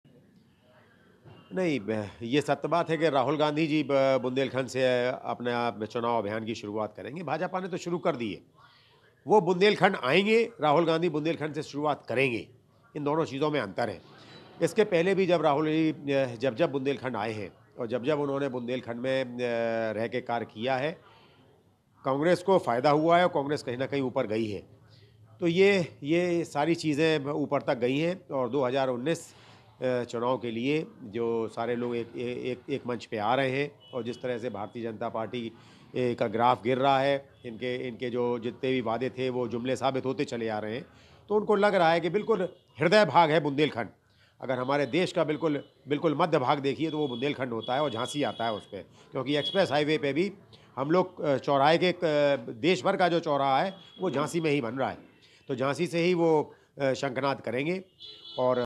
राहुल गांधी बुंदेलखंड की धरती से करेंगे चुनावी शंखनाद
झांसी : बीजेपी पहले से बुंदेलखंड की धरती से शंखनाद करने की मंशा बना ली है. वहीं अब राहुल गांधी भी अपना चुनावी शंखनाद बुंदेलखंड से करने जा रहे हैं. इसके लिए कांग्रेसियों ने ललितपुर में उनकी रैलियां कराने की रणनीति तैयार की है. जनवरी और फरवरी के बीच यहां उनकी रैलियां हो सकती है. राहुल के दौरे से पहले 27 जनवरी को कांग्रेस के वरिष्ठ नेता पी एल पुनिया और मध्य प्रदेश और राजस्थान से कई दिग्गज कांग्रेसी नेता झांसी में जुटेंगे.